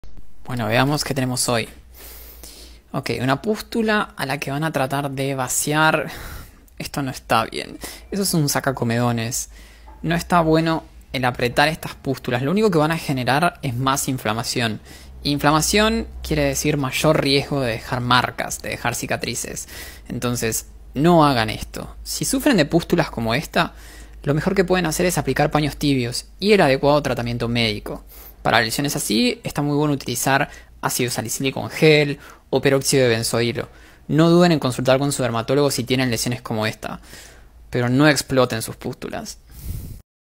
Como NO tratar los granos con pus - Recomendación de un Dermatólogo
¡Este tipo de procedimientos es un riesgo! Genera inflamación y por ende, probablemente deje marcas. Pústulas como estas no requieren ser explotadas con un sacacomedones, requiere tratamiento médico. La idea de explotar un granito es muy tentadora pero... no es aconsejable.